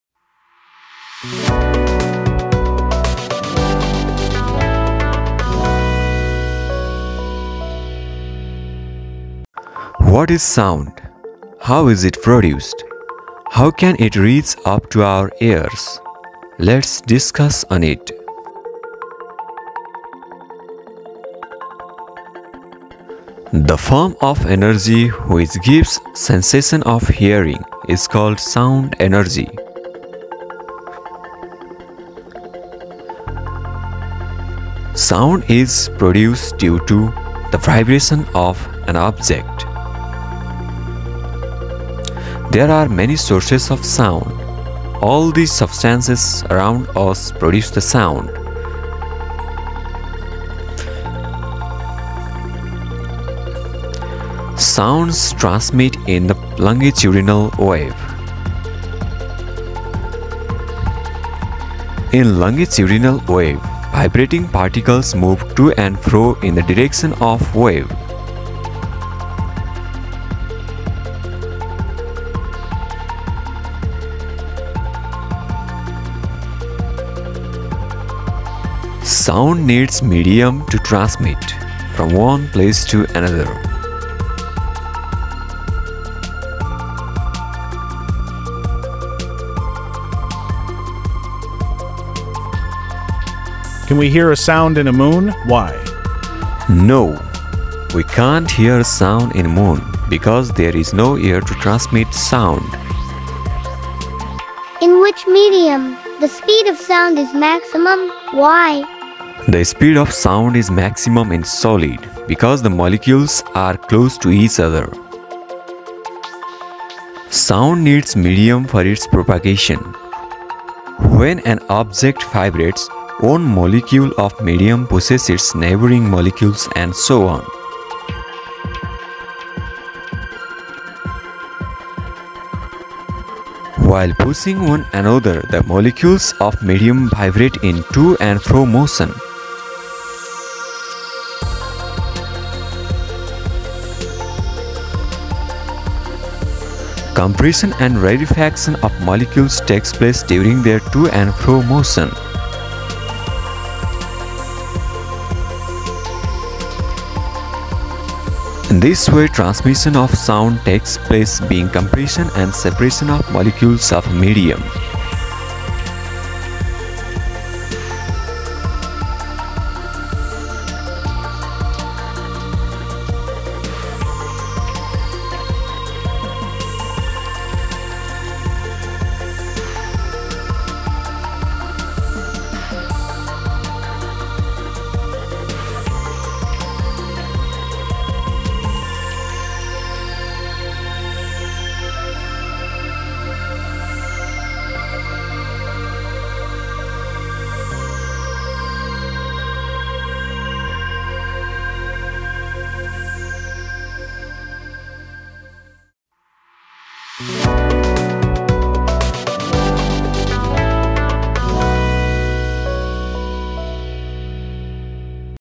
Sound energy , Sources of sound and Propagation of sound waves
What is sound energy? How is sound produced? the propagation of sound and sources of sound